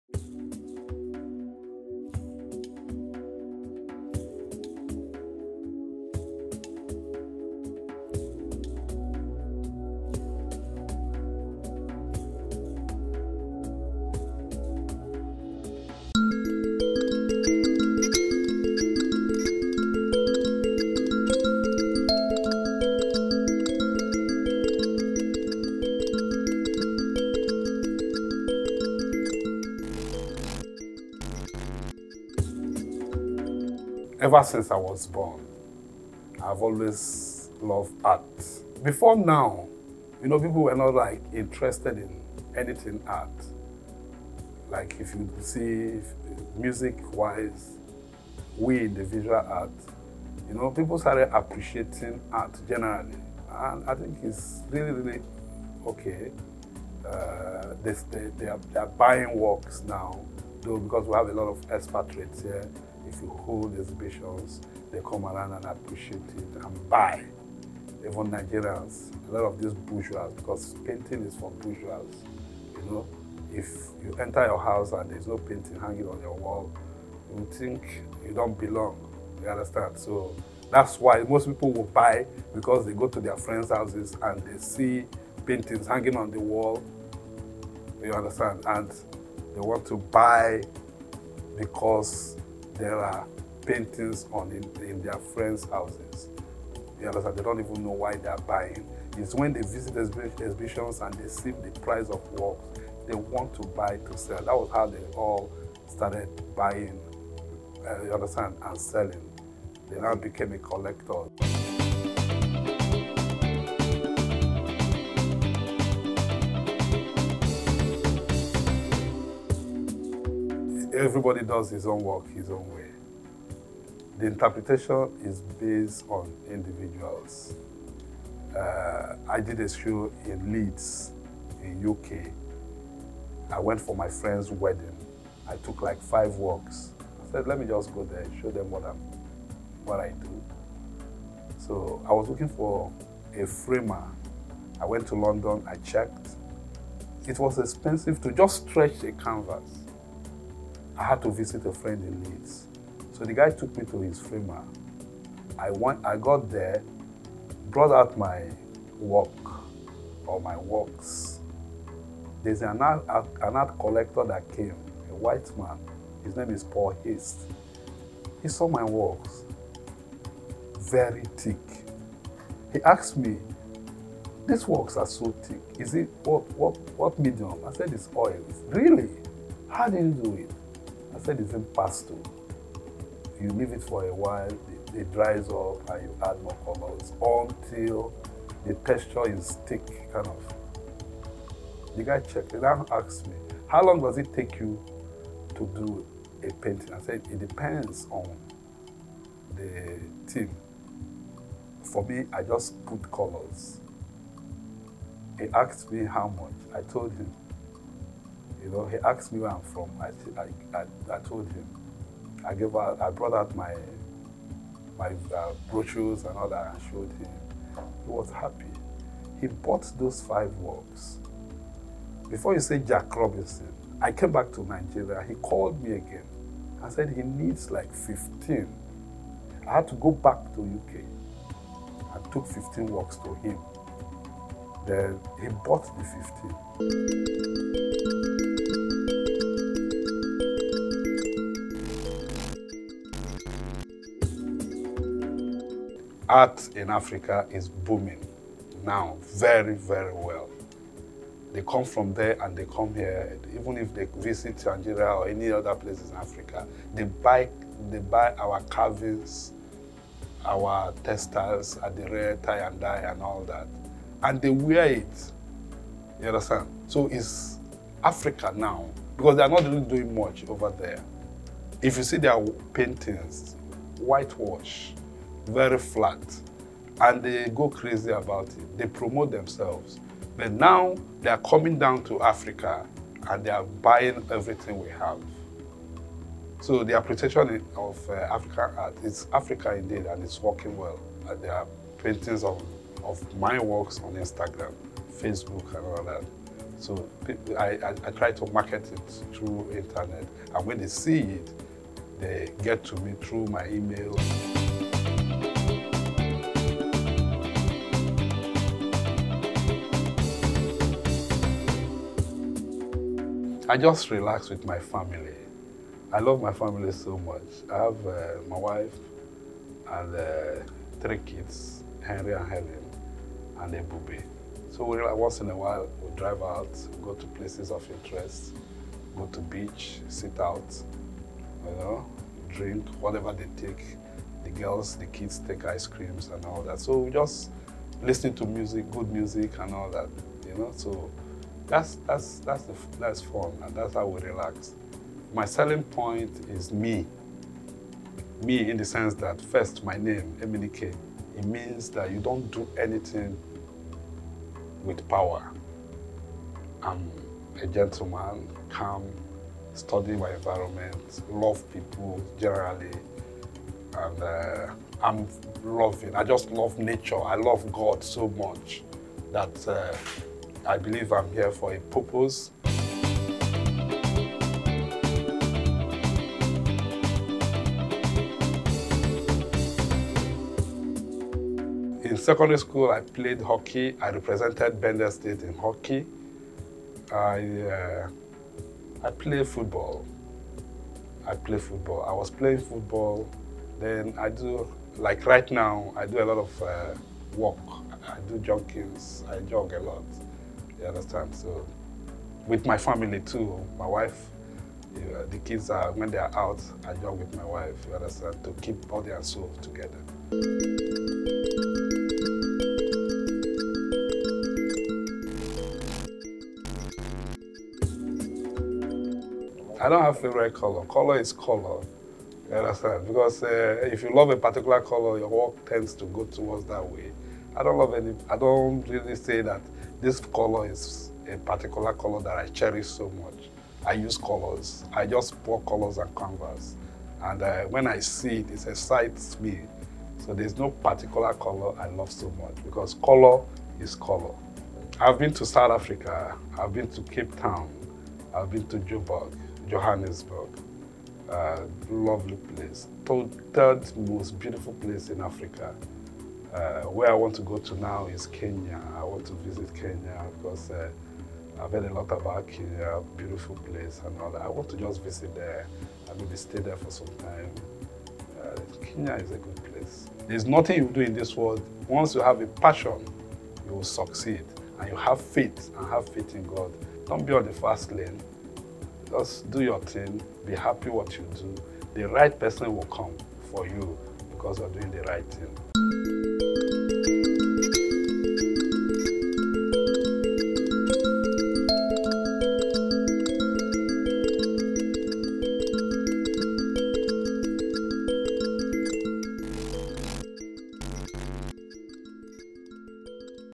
Behind The Magic With Visual Artist Emenike Ogwo | 22/06/2023
Emenike is a veteran artist who has horned his skills in mixed medium and tells stories mostly through abstract works.His works have great appreciation in Europe and across Africa.His dynamic use of the pasta leaves him among the most complex in the industry.